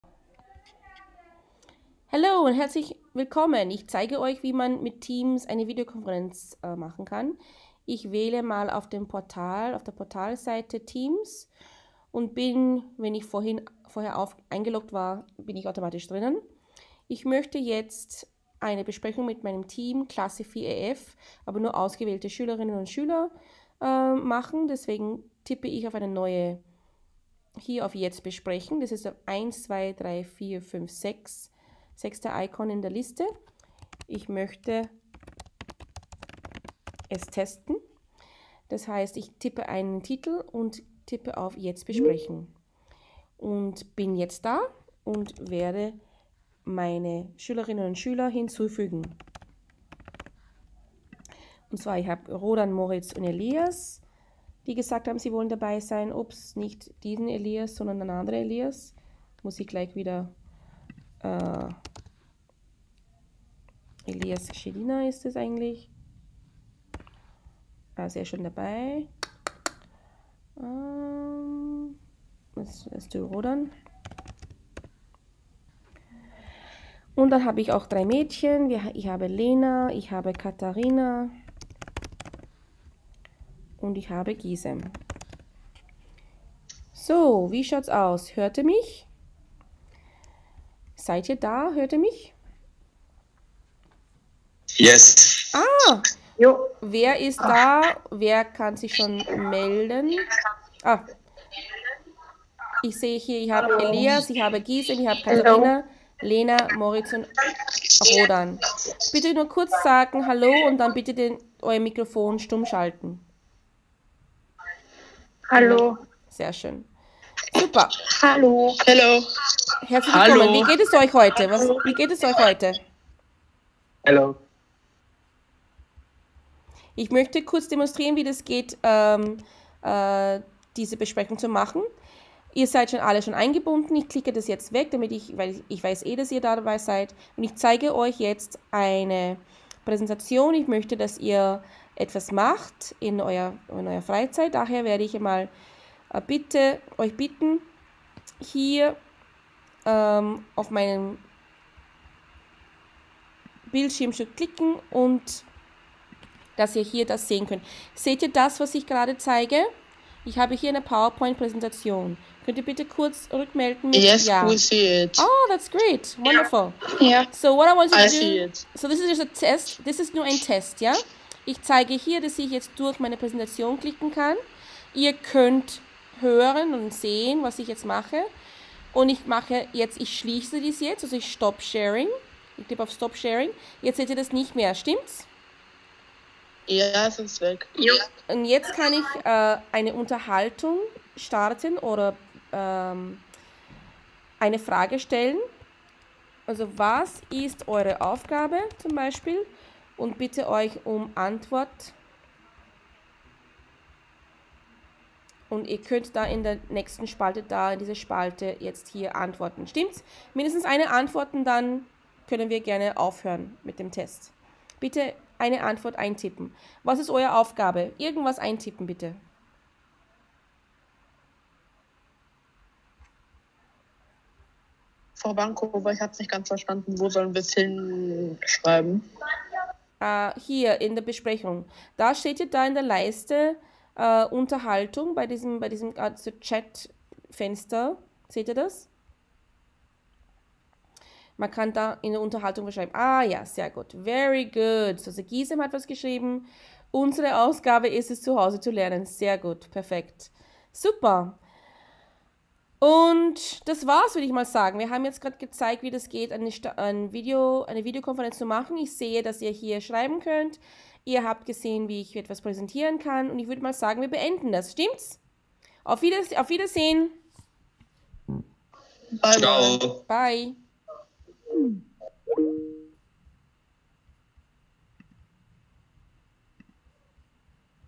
Video Demo: Online Class Meeting with Microsoft Teams
In this video, I demonstrate how to start a video conference with Microsoft Teams and how to share screen.